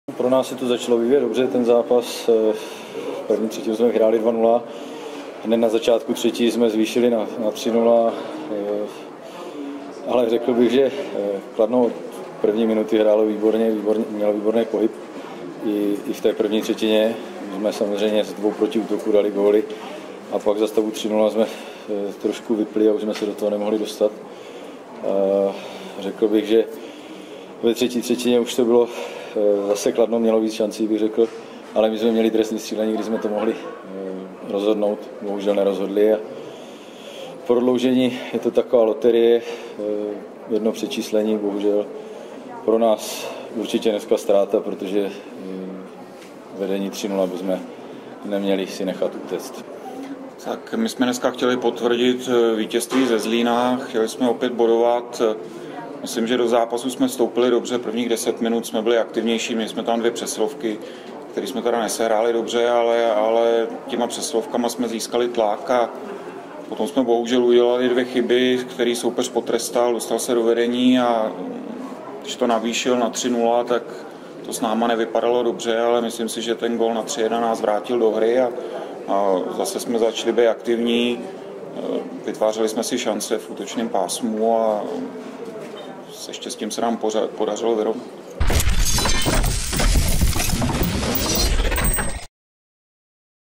Trenéři po zápase: David Čermák a Jan Tomajko
Tisková konference po utkání 6. kola TELH Rytíři Kladno - HC Olomouc 4:3 po prodloužení.